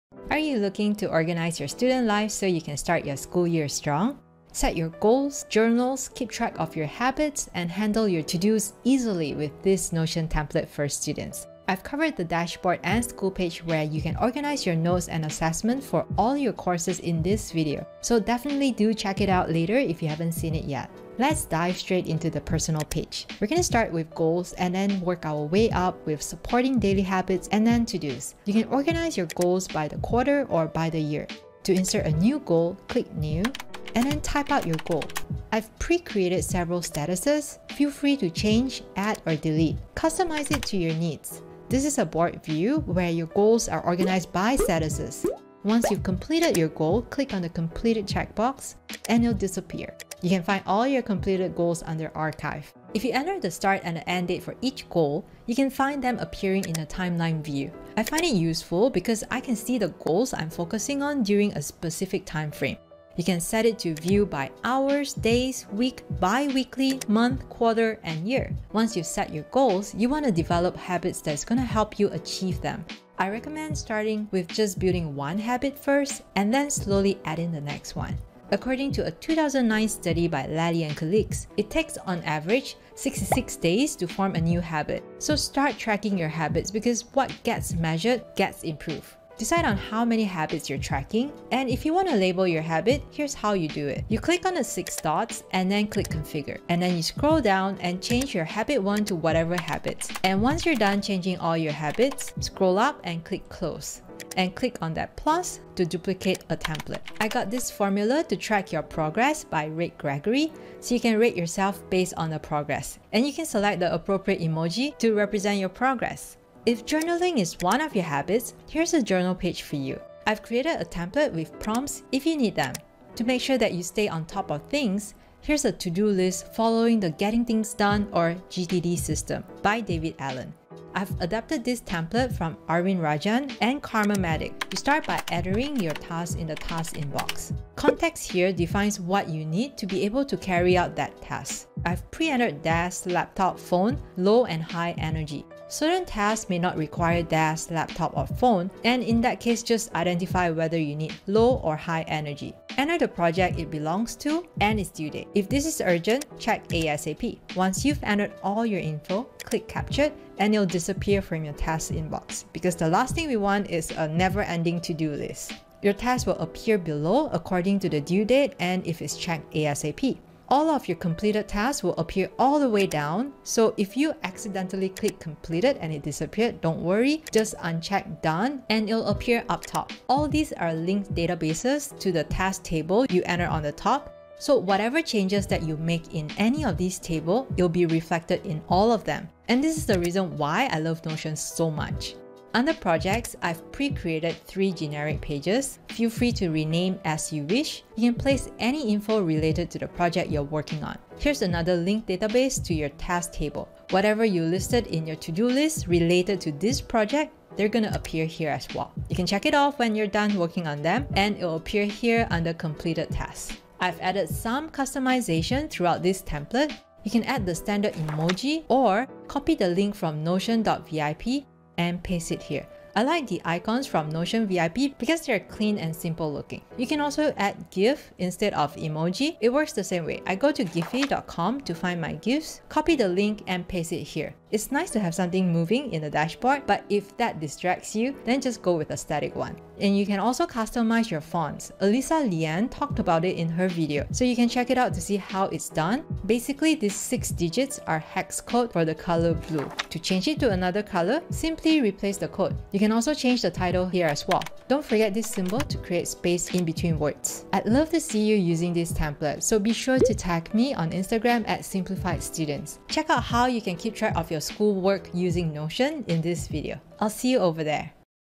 Organize your life as a student 🌟 FREE Notion templates
Are you looking to organize your life as a student? Looking to organize all your notes in one place? Get your To-Dos in order? Track your habits? Never miss a deadline? I've got you COVERED plus so much more! I've created a Notion dashboard that is not only fully functional and practical, but aesthetic as well! Don't you love it when everything is organized and looks pretty? Start your school year strong with this notion templates for students.

🕑TIMESTAMPS:
0:00 Set goals
1:08 Habit formation
1:30 Notion habit tracker
1:51 Notion journal template
2:05 Notion to do list
3:34 Notion customize

🔎 RESEARCH:
Lally, P., van Jaarsveld, C. H. M., Potts, H. W. W., & Wardle, J. (2009). How are habits formed: Modelling habit formation in the real world. European Journal of Social Psychology, 40(6), 998–1009.

QUESTION❓
Have a question about exam preparation, study tips and strategies, or anything else? Post in comments section of this video!

ABOUT:
In this video, we're going on a Notion Tour and Notion Tutorial so that you can start your school year strong using Notion for students. I've created these Ultimate Notion templates for students just so that you can organize your life as a student! It's created with you in mind. Set all your goals. Track your habits. Journal your daily thoughts. Place all your To-Dos  so you won't miss any deadlines! Organize your notes and school stuff plus so much more!